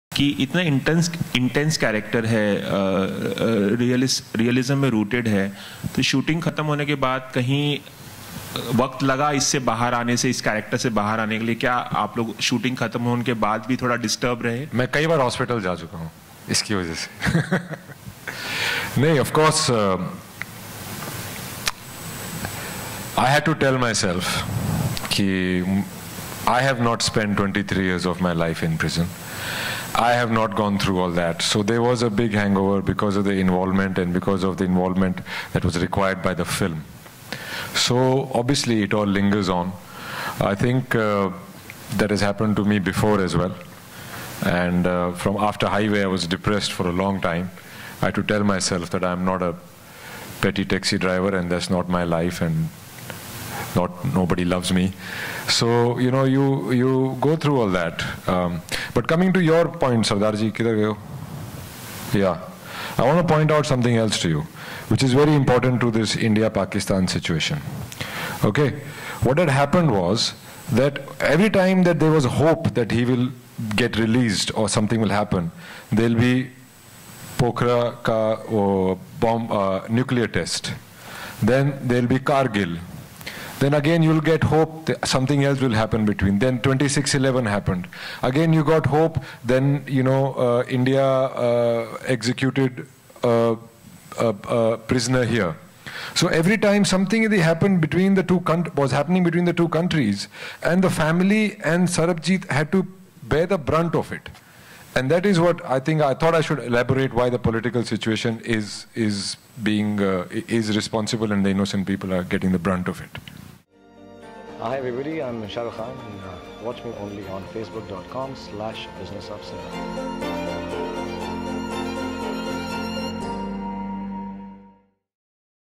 Randeep Hooda Speaks Up On How India-Pakistan War Was Major Cause Of Sarabjit Singh's Tragedy!
Bollywood actor Randeep Hooda along with the star cast of the upcoming film Sarbjit attended the tribute of the late Sarabjit Singh. The actor plays the role of Sarabjit Singh in the film for which he invested a lot of time on his physical look. The actor opened up about how difficult the journey has been. He talked about how Pakistan-India battle affected many a times in his release. Watch to know more!